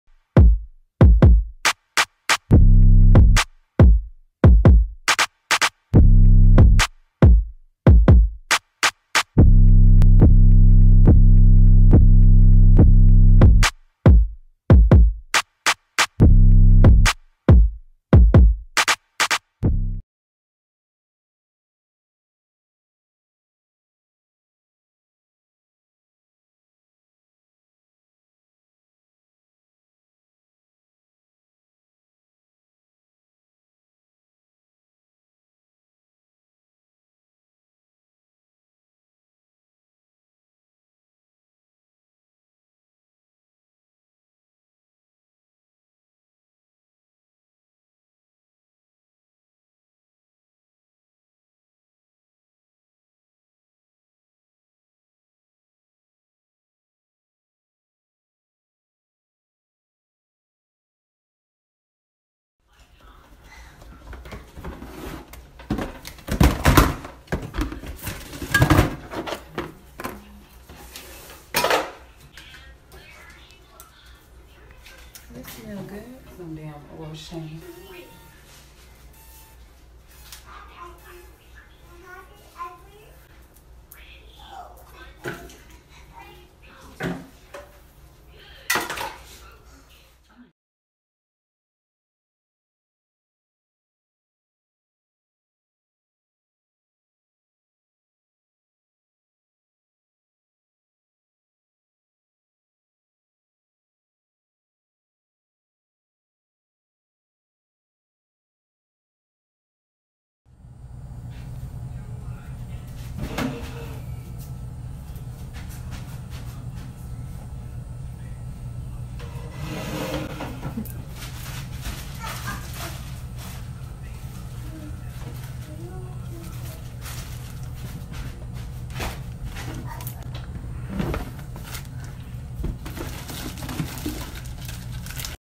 24in 1b/27 ombré Lace wig from Amazon
Hey Deeva Gang here’s another brief tutorial of me applying my lovely friend’s lace wig glue purchased from @vaerosecollection on Instagram make sure you guys shop she has literally everything . Also three layers was used for this melt down ladies get into make sure to leave me some feed back 💙